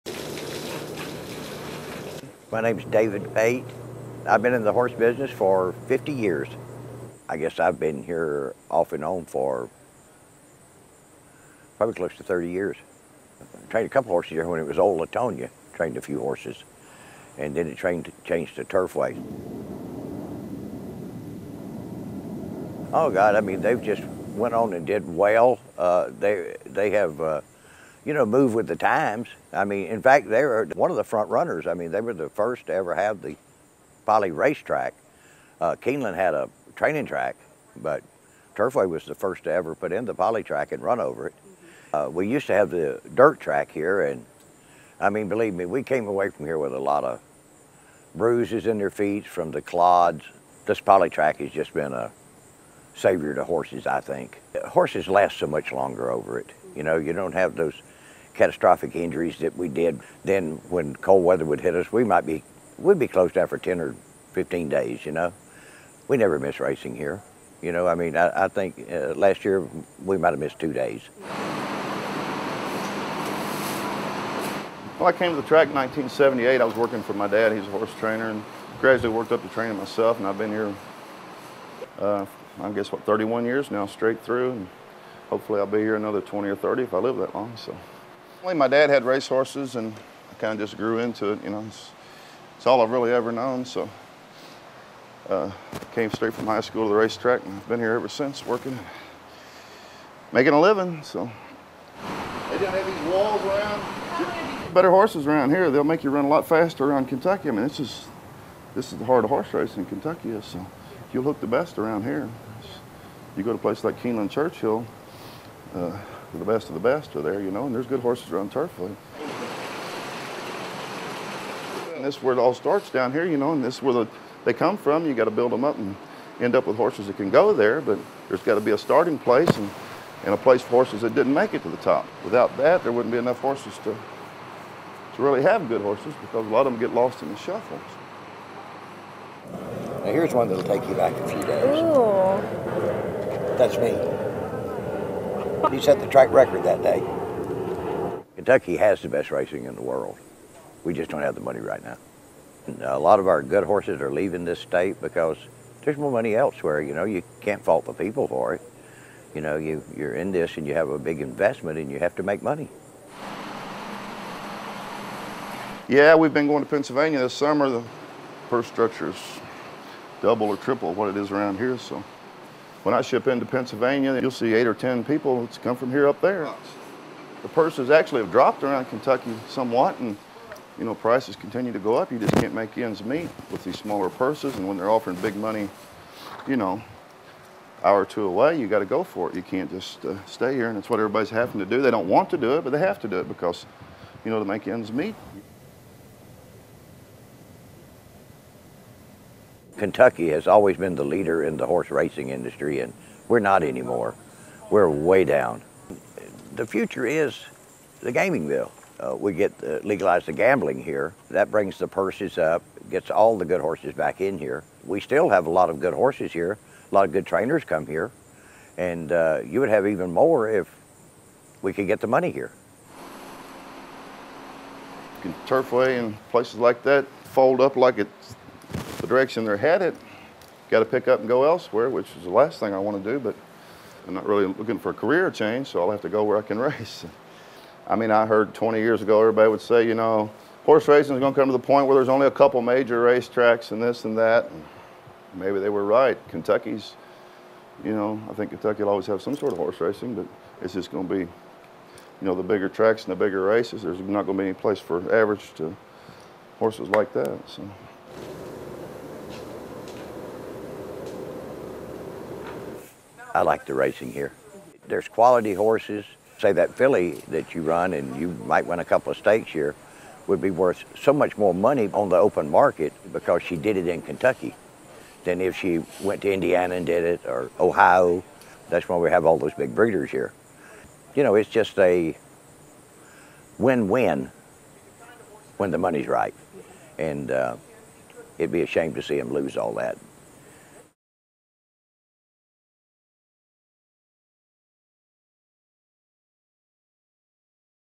Local jockey describes profession
A jockey at Turfway Park, describes his job. Visit nky.com for more.